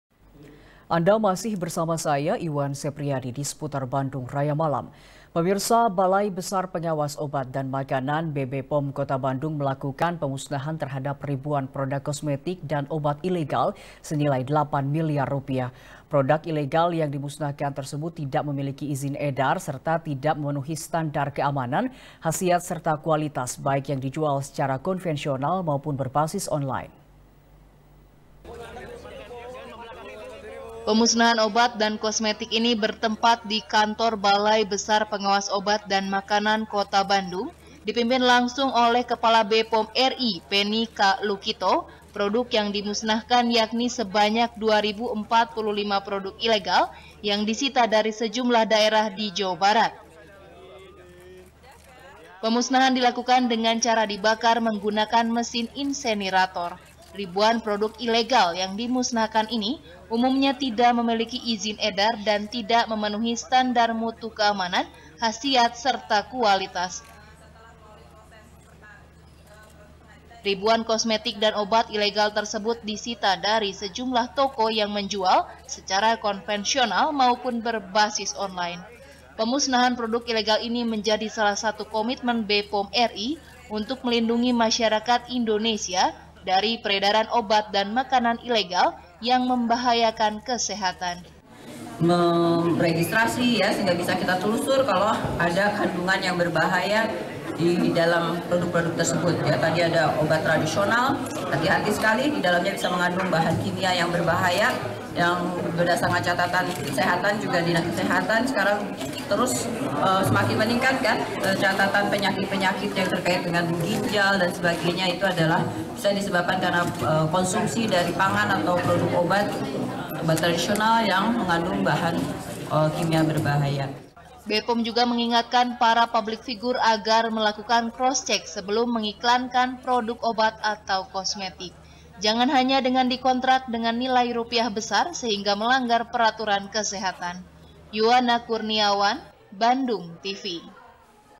BBPOM Musnahkan Obat Dan Kosmetik Ilegal Senilai 8 Milyar | SBR | BANDUNGTV
SBR | BANDUNGTV | IWAN SEPRIADI | Balai besar pengawas obat dan makanan – b-pom kota bandung,melakukan pemusnahan terhadap ribuan produk kosmetik dan obat ilegal,senilai delapan miliar rupiah.produk ilegal yang dimusnahkan tersebut tidak memiliki izin edar serta tidak memenuhi standar keamanan,khasiat serta kualitas,baik yang dijual secara konvensional maupun berbasis online.,